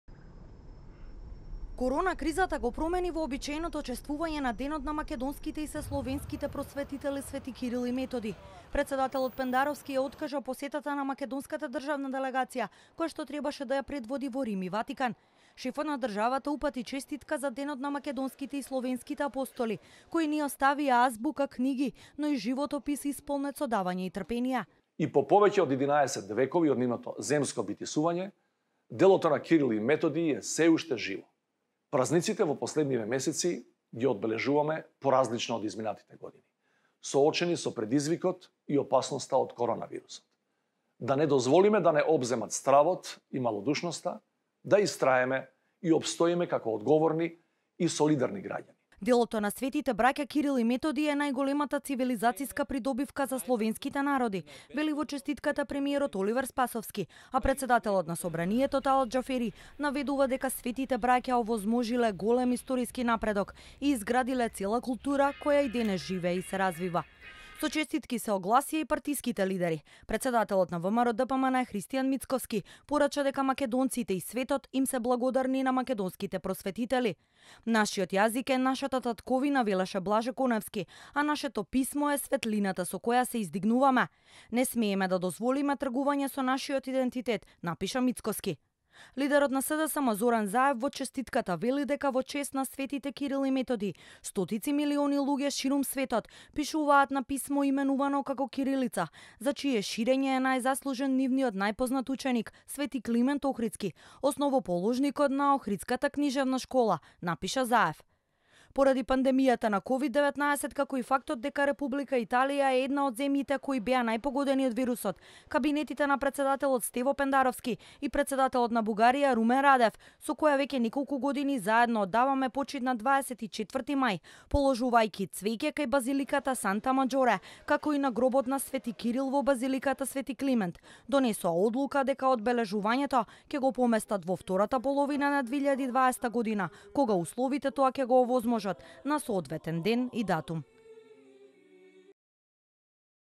Македонија го празнува Денот на македонските и сесловенските просбетители Св. Кирил и Методиј
Македонија го празнува Денот на светите Кирил и Методиј, но годинава поради епидемијата државна делегација не замина на чествувањето во Ватикан.